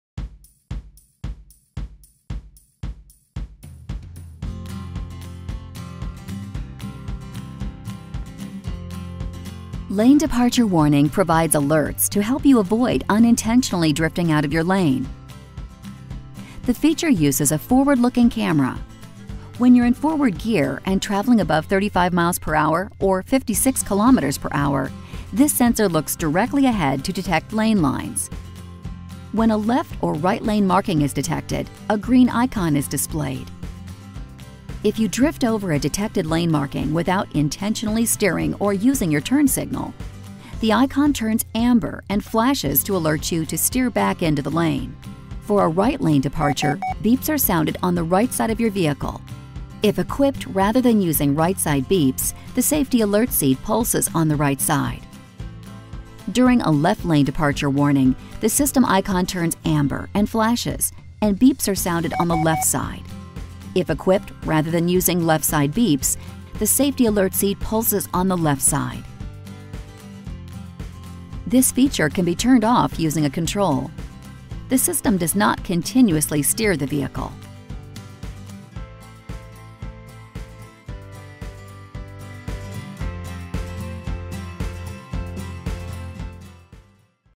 Công nghệ mới cảnh báo xe lệch làn đường
Khi xe đang di chuyển với tốc độ 60 km/h trở lên và lệch làn mà không bật đèn báo hiệu, camera trên xe sẽ phát hiện và cảnh báo bằng hình ảnh và âm thanh .